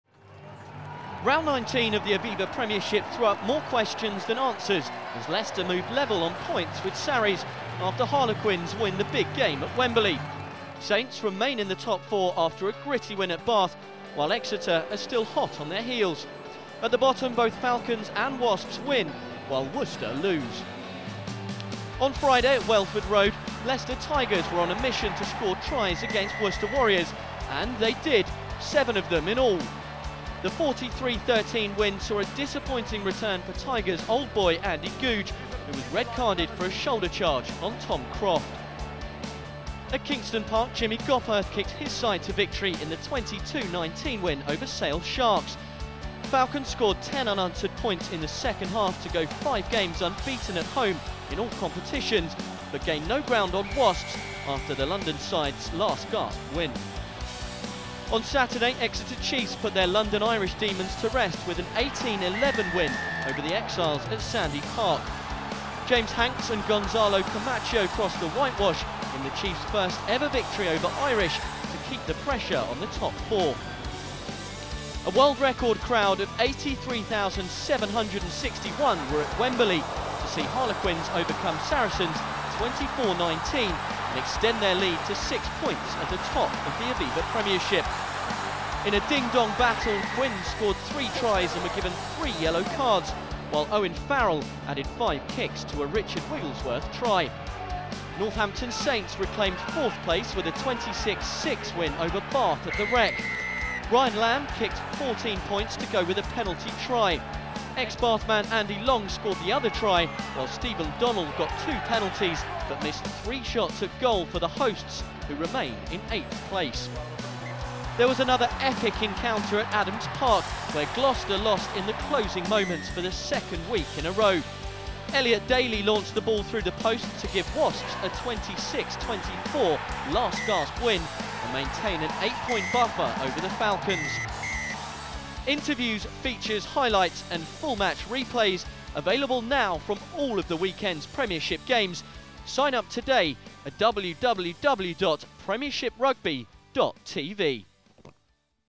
Aviva Rugby Premiership 2011-12 - Round 19 Wrap-up highlights
George Lowe scored one try and saved two others as Harlequins produced an heroic defensive performance to beat Saracens 24-19 in front of a world record crowd and extend their lead at the top of the Aviva Premiership Rugby table.

Lowe is on Stuart Lancaster's list of potential England stars of the 2015 Rugby World Cup and the 22-year-old Harlequins centre proved why on the biggest stage club rugby has ever seen, with a crowd at Wembley of 83,761.
After Jordan Turner-Hall's try had put Harlequins ahead, Lowe produced a top class finish to score his side's second of the afternoon and Danny Care added their third after the break.

Saracens then threw the kitchen sink at Harlequins but Lowe kept the Aviva Premiership Rugby champions out with brilliant try-saving tackles on Michael Tagicakibau and Chris Wyles in the space of three minutes.

Harlequins were under siege and had three men sin-binned - Joe Marler, Care and Nick Easter - as they weathered wave after wave of Saracens attacks.

Richard Wigglesworth finally stole over when Marler and Care were both off the field and Owen Farrell added the conversion to his four first-half penalties.

That was enough to secure Saracens the losing bonus point and move them back up to second place - but Harlequins held out for the victory to open a six-point lead at the top.

This attendance beat the previous world record club attendance of 82,208, which was set in the 2009 Heineken Cup quarter-final between Leinster and Munster at Croke Park.

A total of 165,781 fans have watched the two Aviva Premiership Rugby matches between these London rivals this season, with Harlequins' 'home' game attracting 82,000 to Twickenham.

McFly provided the pre-match entertainment but it was Harlequins who caught the eye immediately after kick-off, with Turner-Hall scoring the opening try after just four minutes.

Nick Evans' clever flick to Tom Williams on the left touchline stretched the Saracens defence and Turner-Hall claimed the inside ball to crash over the line.

Williams' offload looked suspiciously forward but referee Gregg Garner confirmed the try and Evans, who has been in disappointing form with the boot in recent weeks, landed the conversion.

Harlequins were playing more rugby but they were also being penalised heavily by referee Garner, allowing Farrell to land three kicks to nudge Saracens ahead.

Things could have been worse for Harlequins had it not been for another sensational try-saving tackle, this time from the lock George Robson on Saracens wing James Short.

Saracens' lead was only temporary. Lowe cut between David Strettle and Ernst Joubert and then wrestled his way through Alex Goode's tackle to score Harlequins' second try.

Farrell and Evans each missed a shot at goal before the England fly-half brought Saracens back to within two points with his fourth penalty.

Harlequins were forced to make a change at the interval with fly-half Rory Clegg replacing Evans. The newcomer managed one penalty from three attempts.

Then Care struck. Brown made the half break and offloaded to Tom Guest, who charged into the 22, drew Alex Goode and sent the exiled England scrum-half over for the try.

Saracens then laid siege on the Harlequins line, forcing the Aviva Premiership Rugby leaders into lung-bursting defence for almost all of the final 20 minutes.

Goode's elusive break launched Saracens into the Quins half and Jackson Wray took the ball on to build the pressure but the final pass just escaped Strettle down the left.

Saracens secured the lineout and drove for the line, building the pressure on Harlequins until Marler was penalised and sent to the sin-bin.

Lowe was Harlequins' defensive hero, making those two try-saving tackles to keep Saracens out. The first was on Tagicakibau, when he combined with Williams to drive the Saracens wing into touch after Strettle and Hodgson had combined in midfield.

Still Saracens pounded away and still Harlequins resisted, with Lowe tackling Wyles into touch after Farrell and Strettle had created the overlap.

Harlequins were reduced to 13 men when Care was sent to the sin-bin for running a cynical off-side line and finally Saracens managed to breach the defence.

Saracens opted for the scrum and replacement Richard Wigglesworth stole over the line to tee up a grandstand finish.

Marler returned and was almost immediately replaced in the sin-bin by Easter, who was penalised for pulling a lineout down, but Harlequins survived to claim a significant victory.